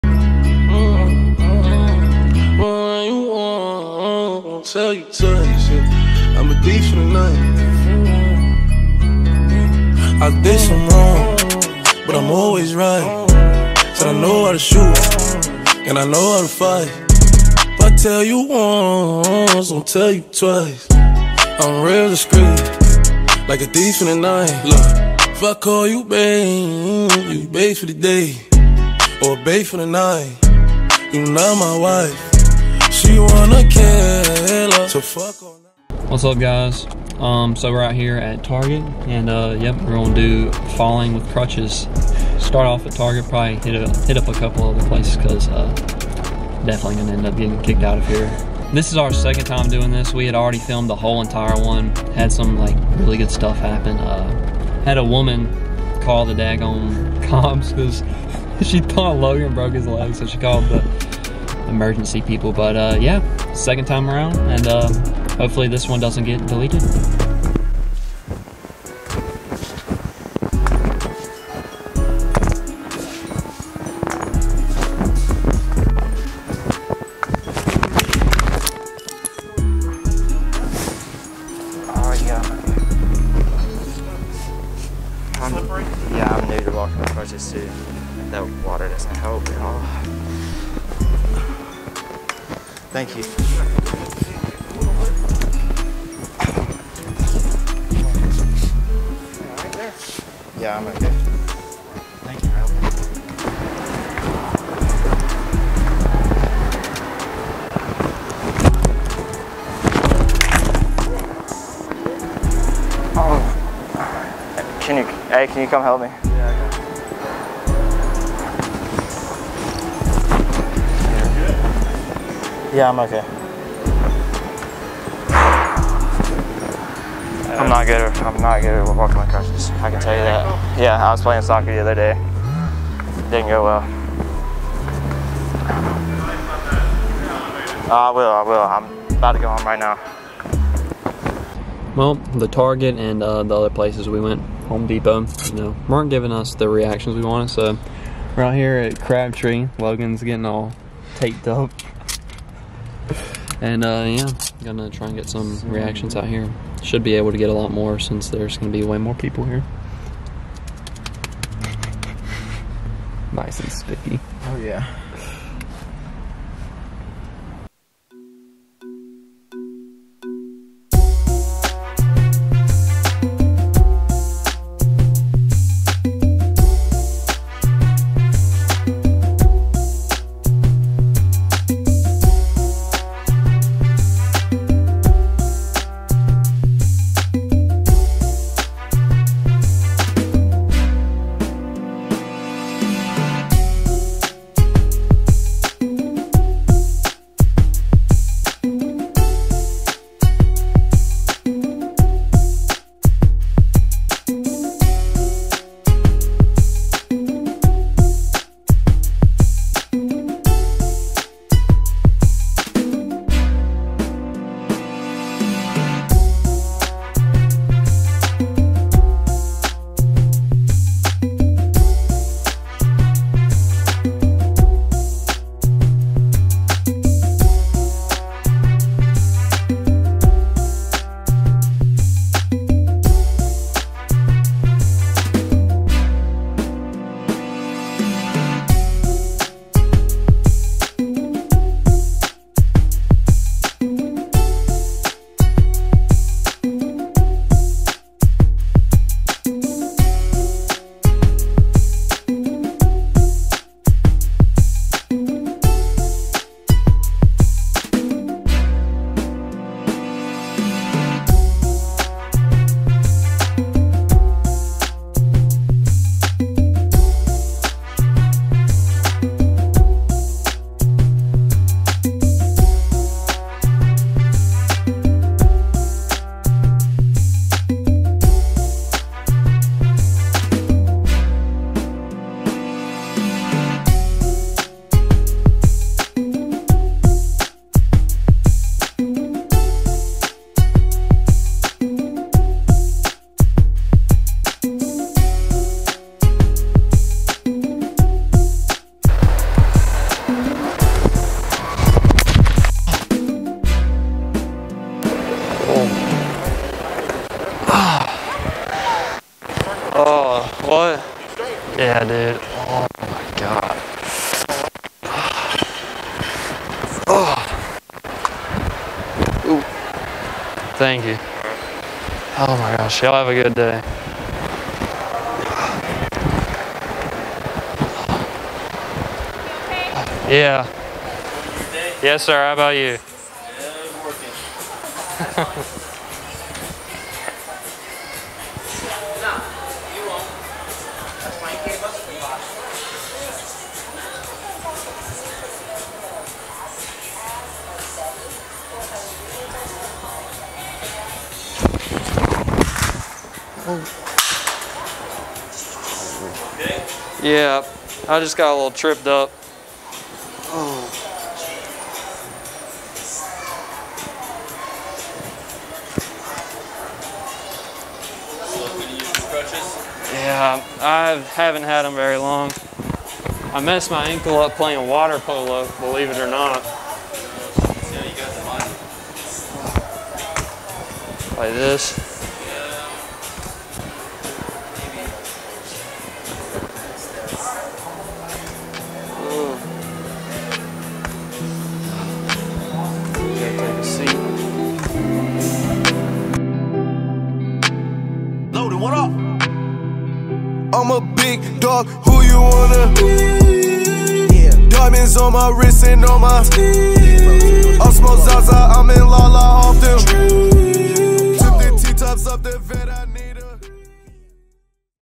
Falling With Crutches Prank
We went to the mall and a couple of other stores and acted like we were falling off of crutches to see people's reactions.

Sorry for the lack of uploads, our SD card messed and we lost two videos we had filmed and ready to go